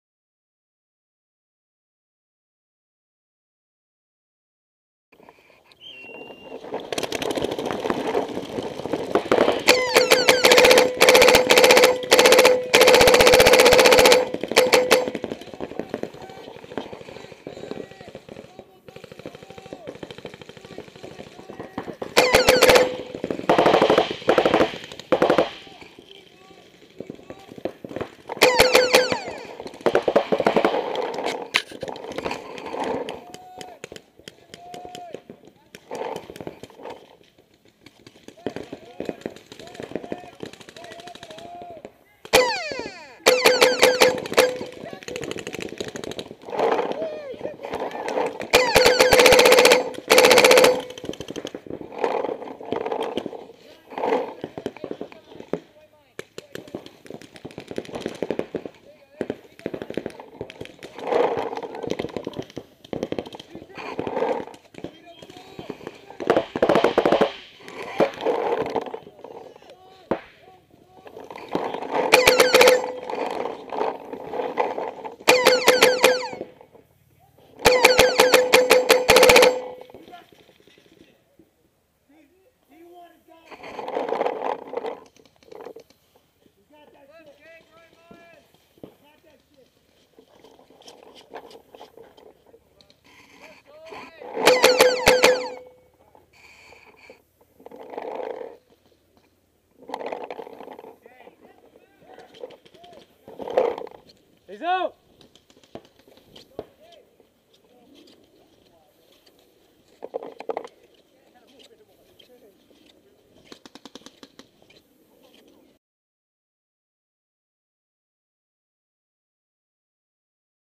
Genesis Paintball Speedball Raw Footage! Etek3 Lt, Barrel Cam HD.
My first 4 pack ever! Barrel Cam footage at Genesis Paintball! That noise you hear everytime I shoot my gun is my LPR? Its been doing that for 3 weeks now, if anyone has any ideas on what it might be in my LPR that is making that noise it would be helpful cause its really annoying! Enjoy the video and let me know what you think?